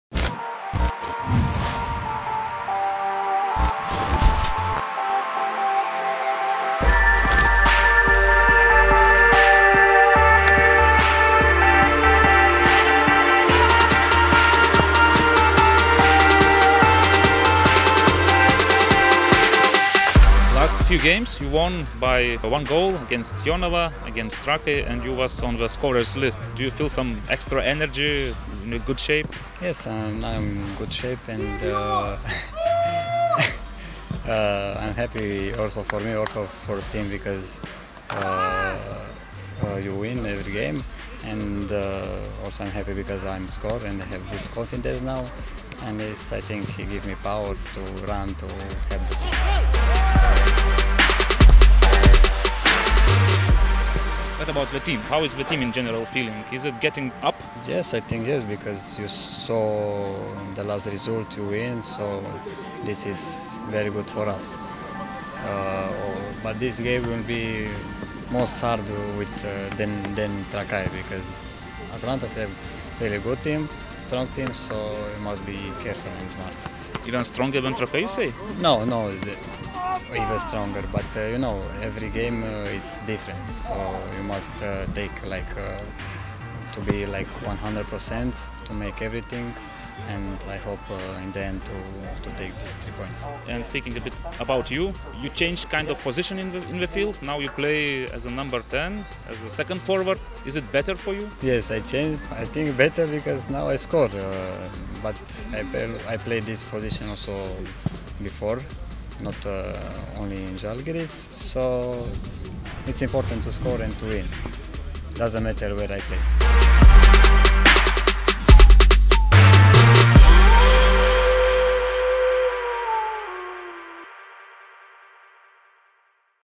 Liviu Antal: „Artėjame prie optimalios formos“
Šį sezoną 6 tiksliais smūgiais pasižymėjęs „Žalgirio“ saugas Liviu Antalis savo įvarčių sąskaitą atsidarė 7 turo rungtynėse Klaipėdoje su vietos „Atlantu“. Šeštadienį 28 metų futbolininkas Vilniuje turės galimybę dar kartą patikrinti jėgas ir sėkmę dvikovoje su uostamiesčio ekipa.